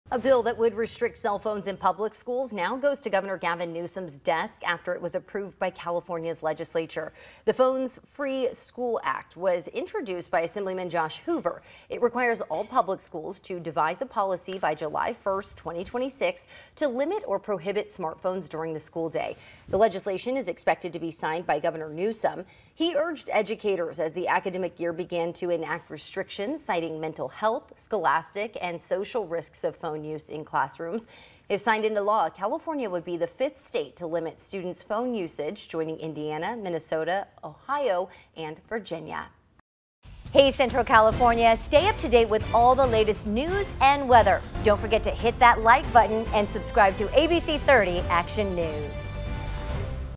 California passes bill requiring schools to devise plan to ban or limit phones
The California legislature passed a bill this week requiring all public schools to come up with a plan to limit or ban phones during the school day, and Gov. Gavin Newsom is expected to sign it into law.